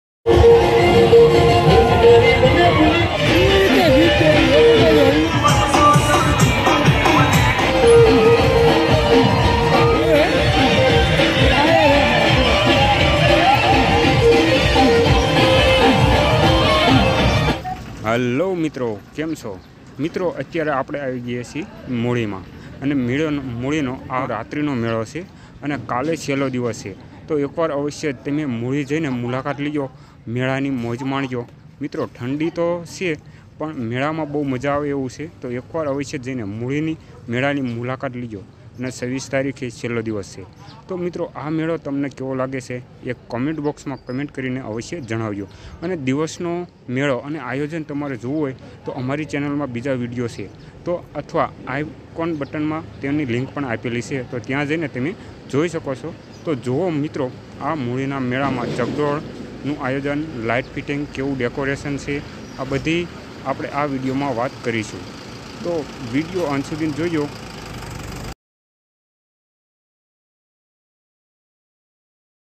મૂળી રાત્રી મેળો 2023||મૂળી મેળો 24/1/2023||સ્વામિનારાયણ ઉત્સવ 2023
મિત્રો આ વિડીયો મા મૂળી સ્વામિનારાયણ ઉત્સવ ના મેળા વિષે બતાવવામા આવયૂ સે જે મૂળી મા લોકોની ભીડ જોવો અને મેળા ની મોજ કરો